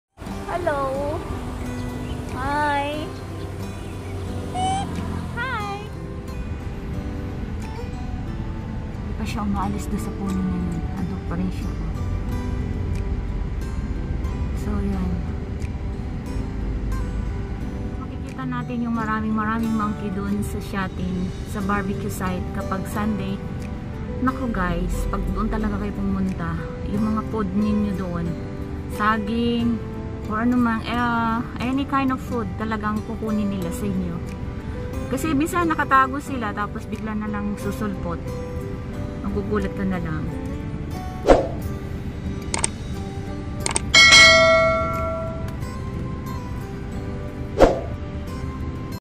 MGA MONKEYS BAWAL SILANG LAPITAN!
bawal silang pakainin at lapitan pero ang cute nila..